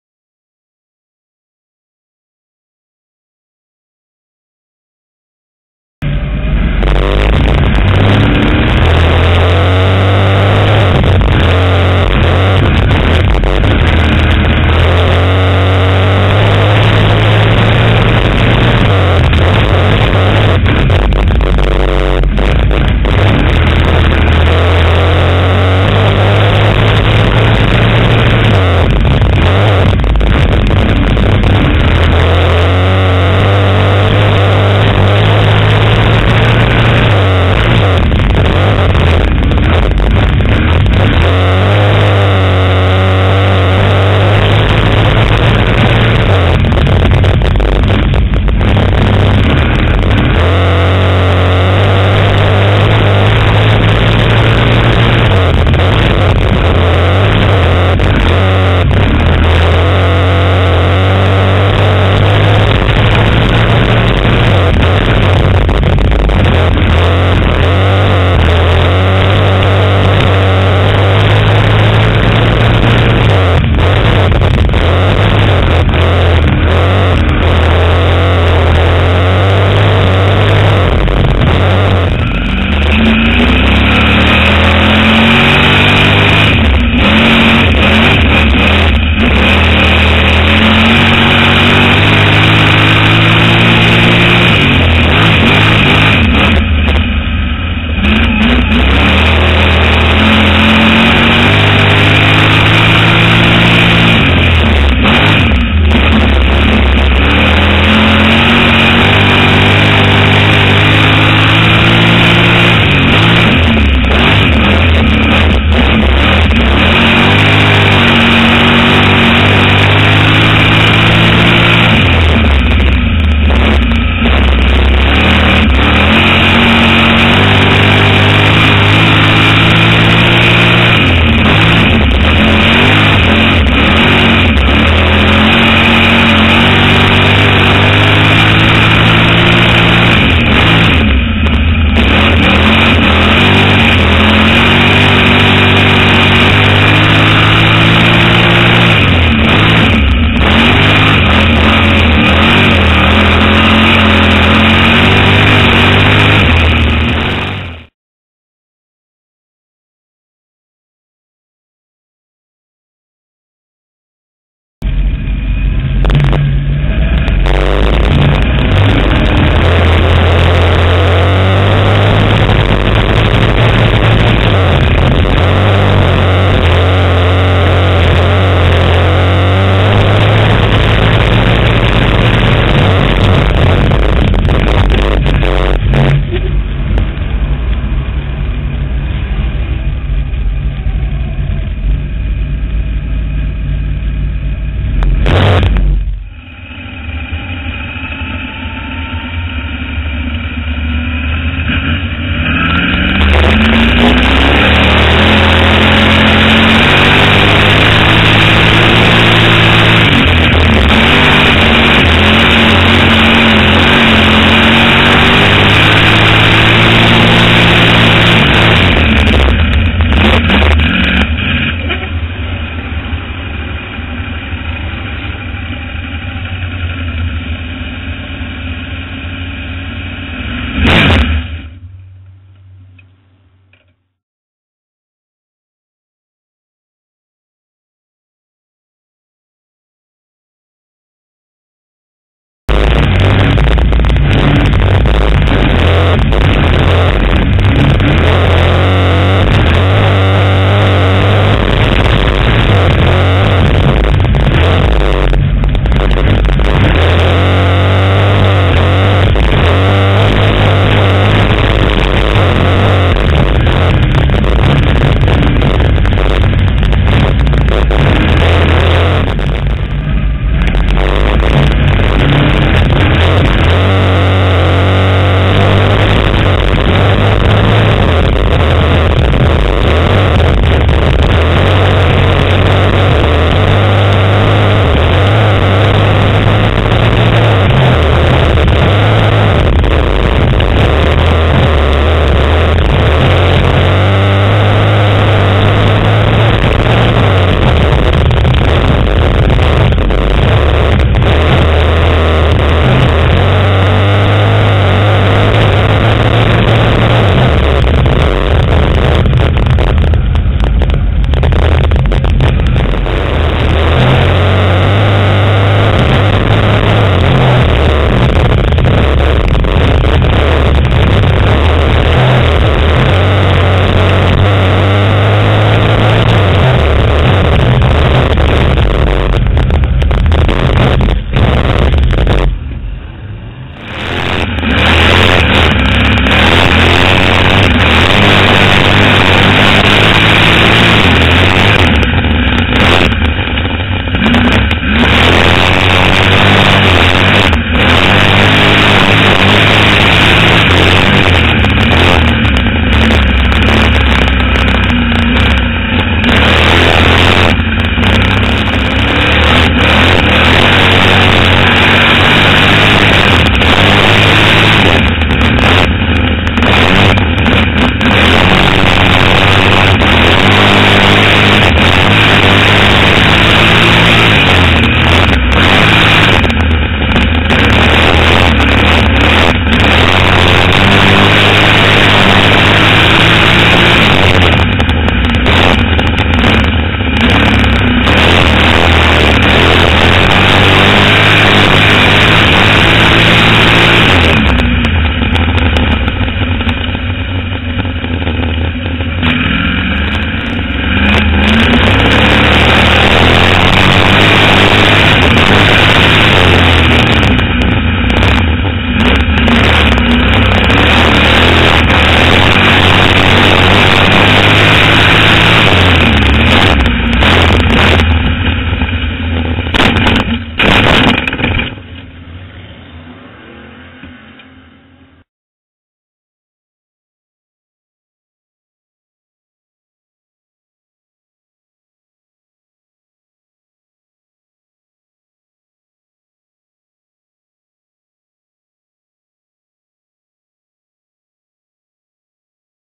Lincoln Speedway 8-7-2010
Incar cameras with 305 driver Billy Ney.